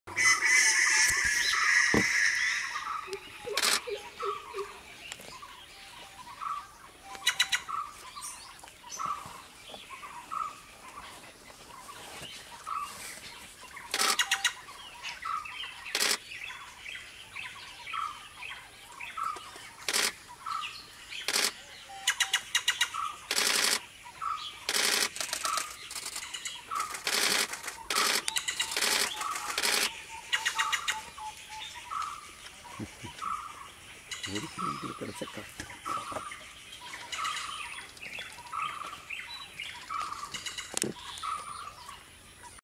White Bellied Treepie
White Bellied Treepie courtship at Thattekad,Kerala